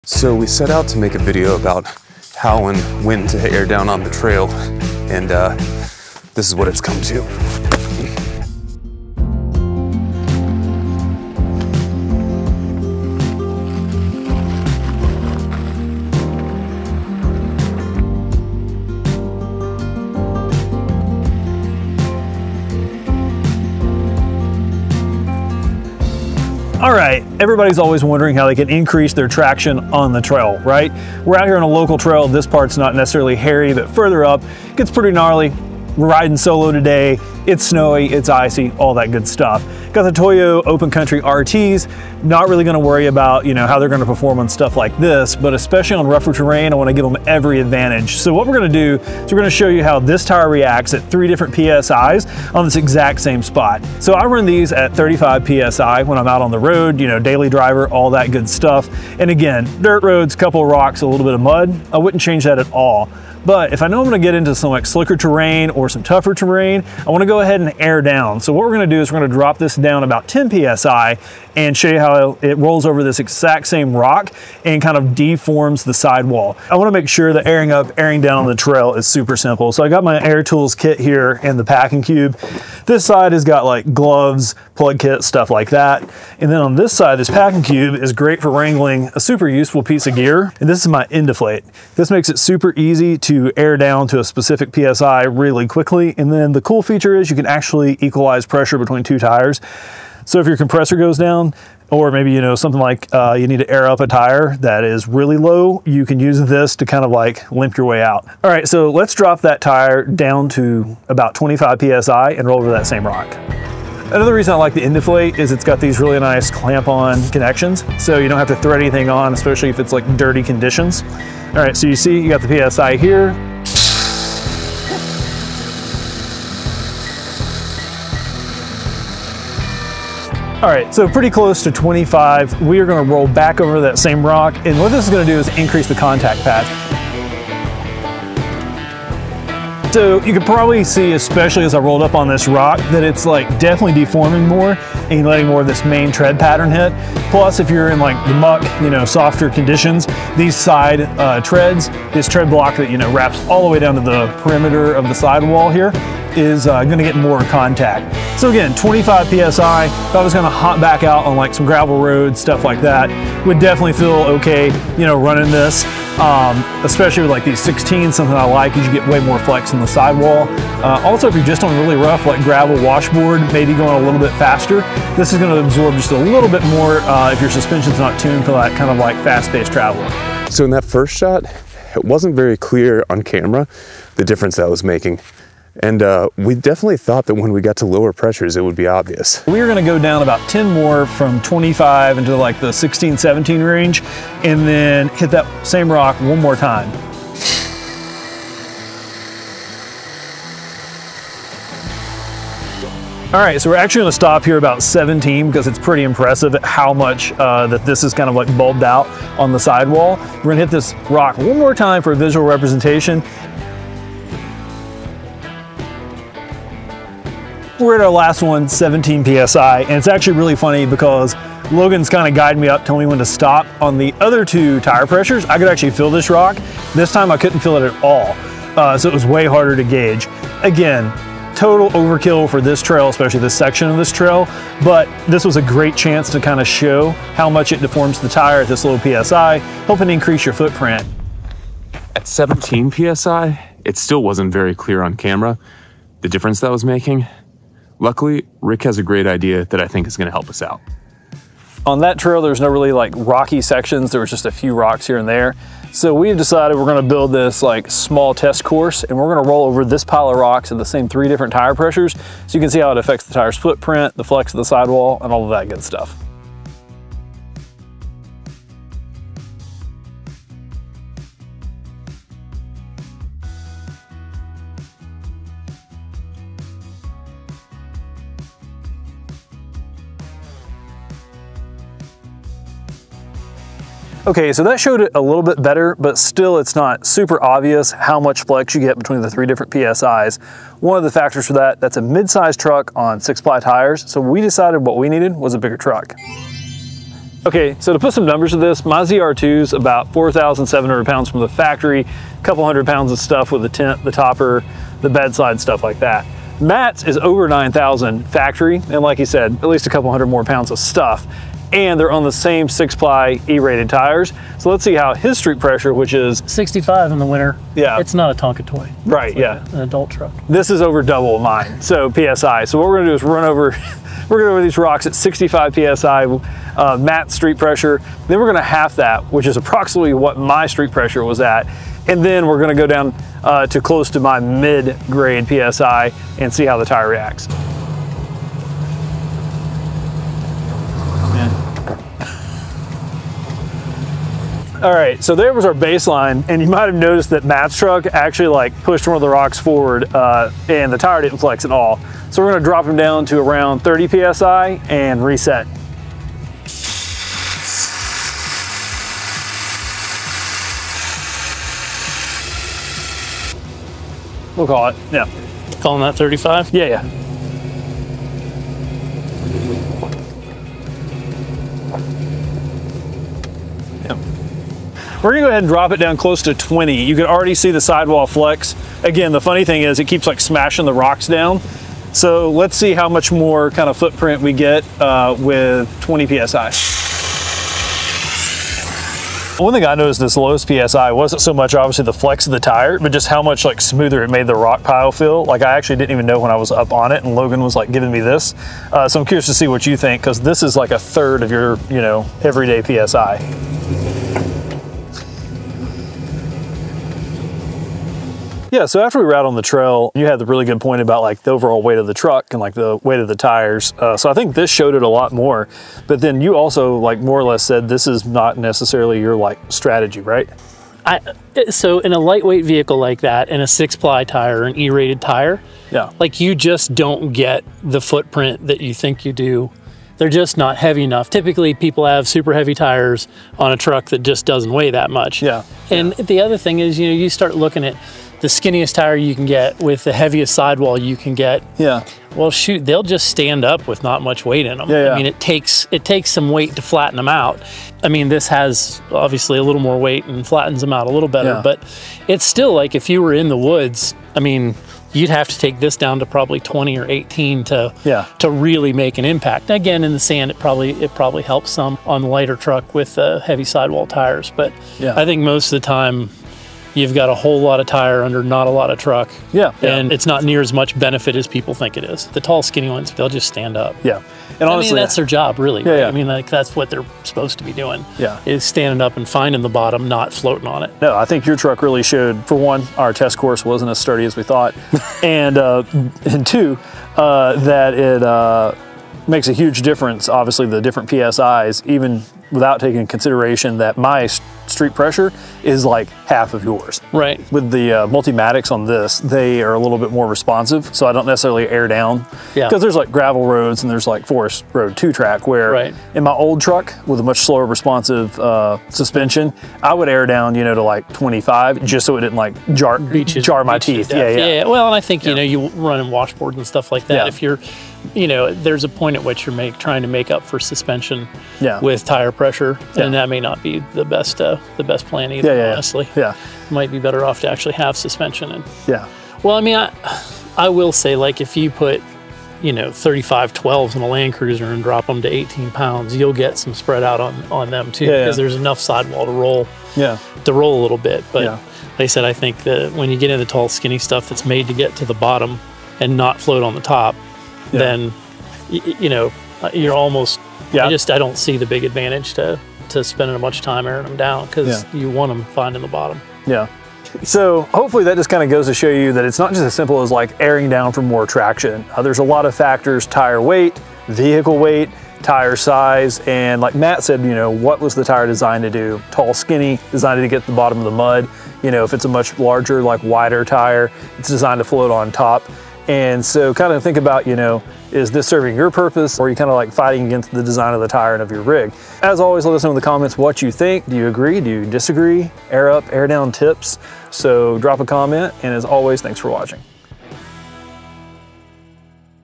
When Should You Air Down? Tire Pressure Tested on the Trail
Airing down your tires is one of the most talked-about off-road techniques — but when does it actually help, and when does it not? In this video, we break down how and when to air down on the trail, using real-world testing to show what actually changes when tire pressure drops.

We start by demonstrating tire deformation and contact patch differences at 35 PSI, 25 PSI, and 15 PSI, showing how lower pressure increases sidewall flex and footprint when crawling over rocks. Our initial test didn’t translate well on camera, so we adjusted the plan, built a more visible rock course, and brought out a larger, heavier truck to clearly show what’s happening at each pressure.

Along the way, we also talk about air deflate tools, why they make pressure adjustments easier on the trail, and how vehicle weight, tire construction, and tire size play a huge role in whether airing down actually improves traction.

One big takeaway? Airing down isn’t a magic traction fix. Its effectiveness depends on your tire’s design, your vehicle’s weight, and what you’re trying to accomplish. In some cases, the difference is subtle — and that’s important to understand before dropping pressure too far.

In this video:

How airing down affects tire shape and contact patch

35 PSI vs 25 PSI vs 15 PSI on rocky terrain

Real-world rock pile testing

When airing down helps — and when it doesn’t

Why tire and vehicle design matter more than hype

Using air deflate tools on the trail

If you’re diving into off-road driving or want a better understanding of what’s actually happening when you air down, this video will help you make smarter decisions on the trail.

👉 Drop a comment with your go-to tire pressures